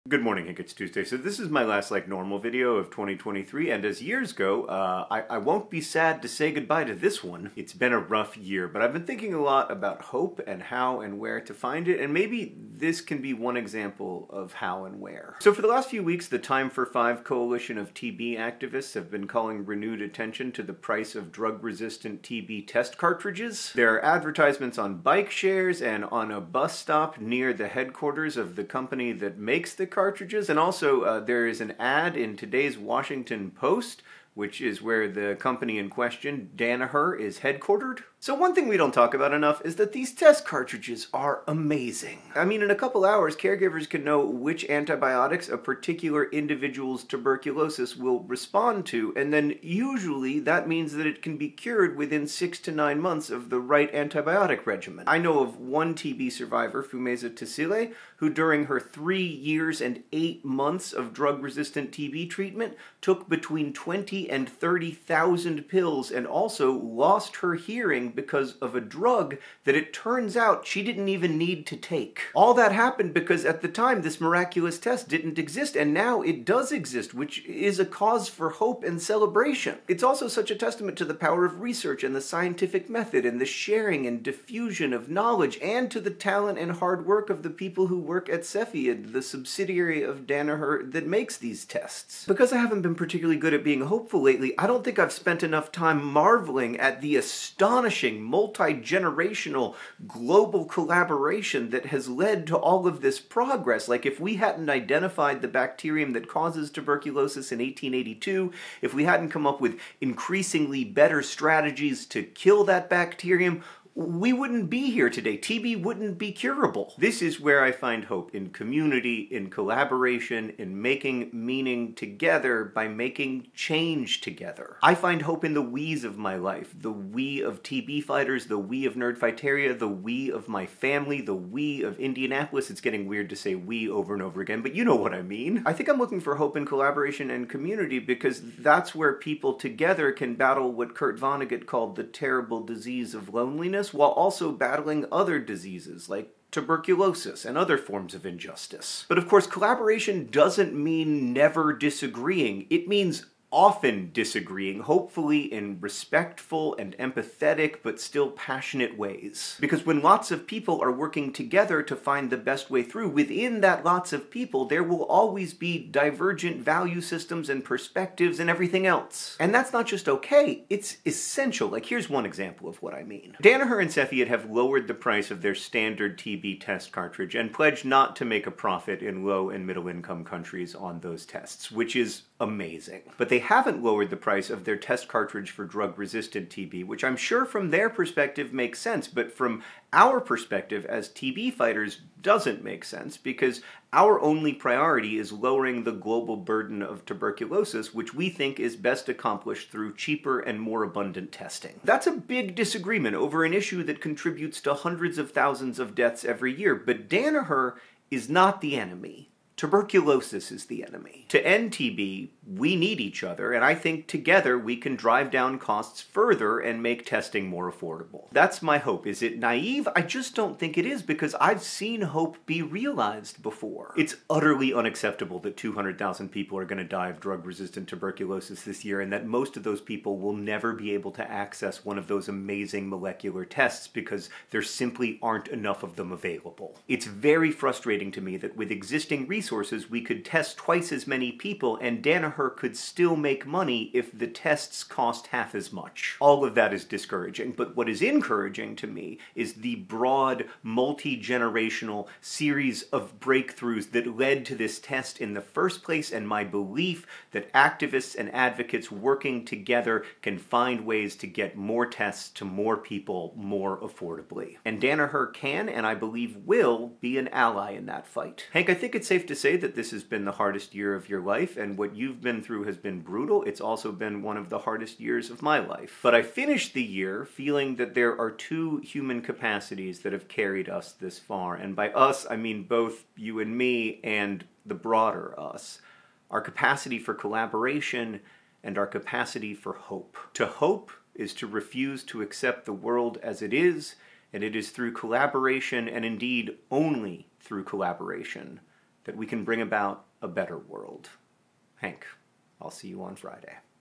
End of a Rough Year
In which John ends 2023 both looking for hope and trying to define it.

Danaher, it's timefor5. Together, we can end tuberculosis.